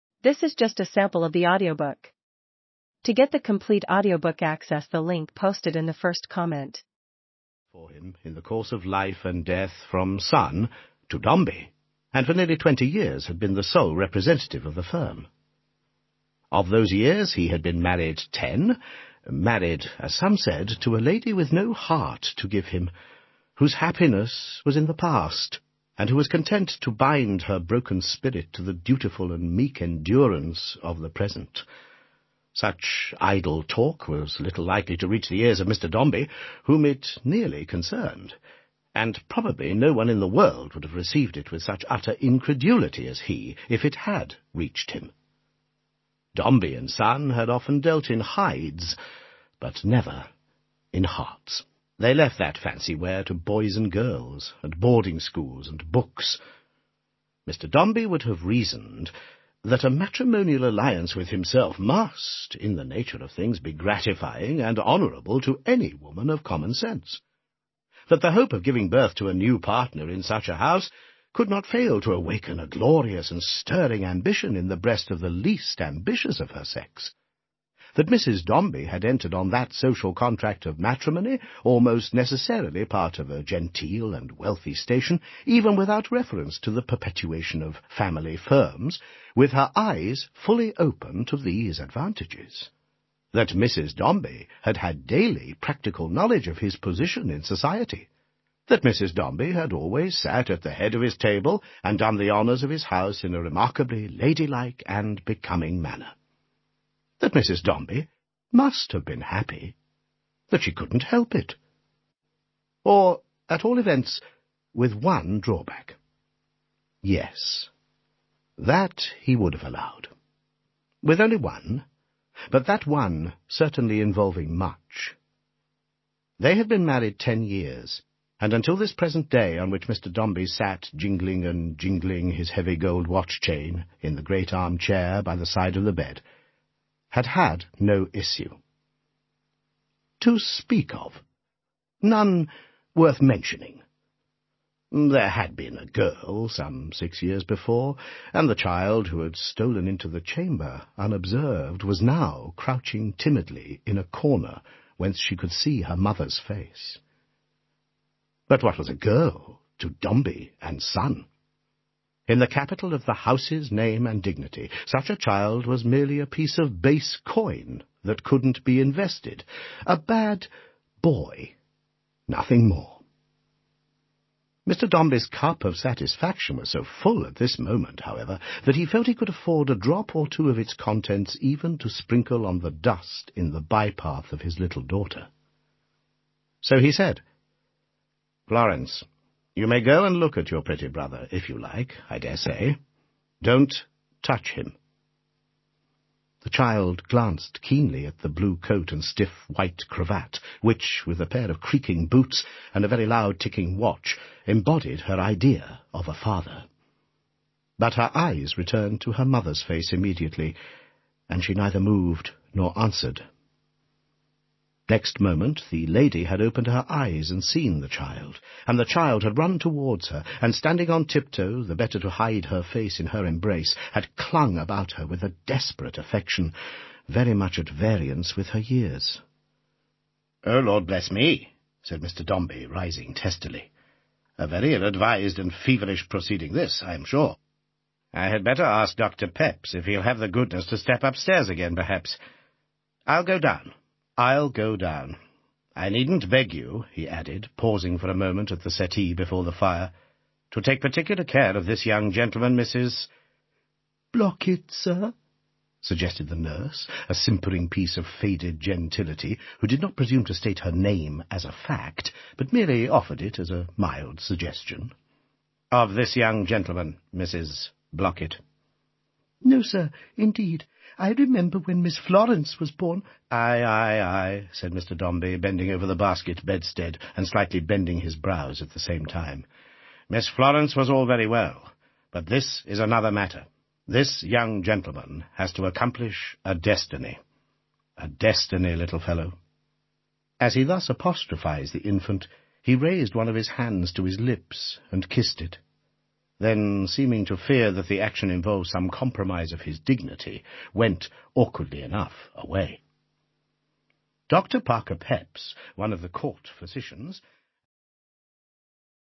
Dombey and Son | Audiobook Sample
Dombey and Son

Dombey and Son is vintage Dickens and explores the classic themes of betrayal, cruelty and deceit. Dombey's dysfunctional relationships are painted against a backdrop of social unrest in industrialized London, which is populated by a host of fascinating and memorable secondary characters. The complete and unabridged novel is brought spectacularly to life by veteran reader David Timson.